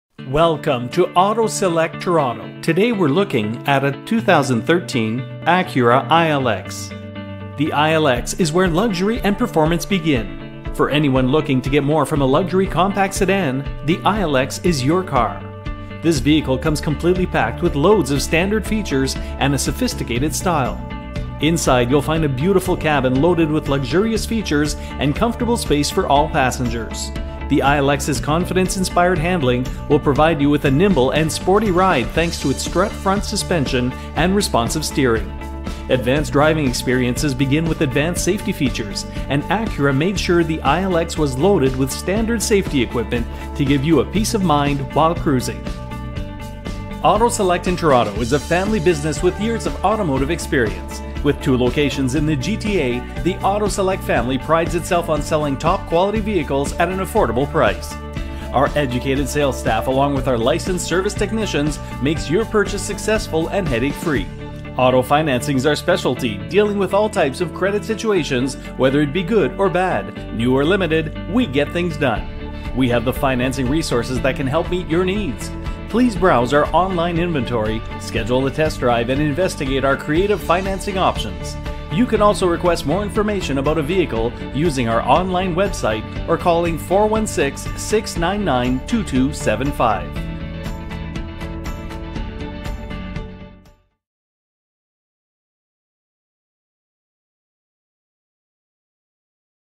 2013 Acura ILX | Auto Select Toronto | 19VDE1F52DE401177
This 2013 Acura ILX Premium Pkg with only 60,485 Kilometers is a one owner Premium vehicle that just came in and is priced for a QUICK SALE. The Premium Pkg on this New Body Style Acura ILX includes Back-up Camera, Bluetooth, Paddle Shift, and Cruise Control! This 2.0L 4 Cylinder Acura ILX has plenty of power and is fuel efficient too...

Preferred and CLEAN colour combination of Titanium Grey with Black Dakota Leather, this Automatic 4 door Luxury sedan is absolutely FLAWLESS inside and out. Sharp look with Xenon Lights, Fog Lights, and 17 inch Alloy Wheels!! Other premium options on this Premium ILX include a Sunroof, Heated seats, and Push Start! Also comes with a CLEAN Carproof Report with no accidents!!!